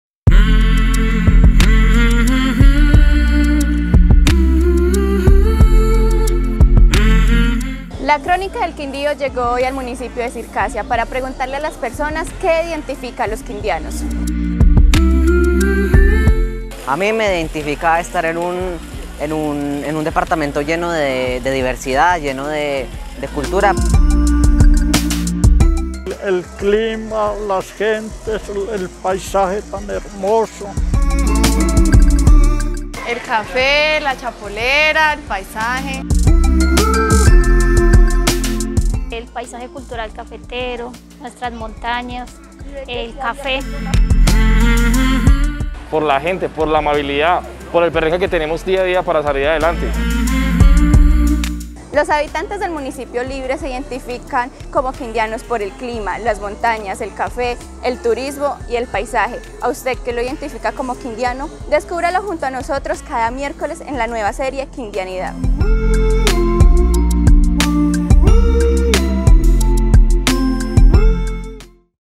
Cronicápsula | ¿Qué identifica a los quindianos?
La Crónica del Quindío recorrió las calles del municipio libre del departamento, preguntándole a sus habitantes ¿qué lo identifica como quindiano?

 Espere cada miércoles, en la edición impresa y página web la serie Quindianidad, y descubra  un poco más de la identidad de la región.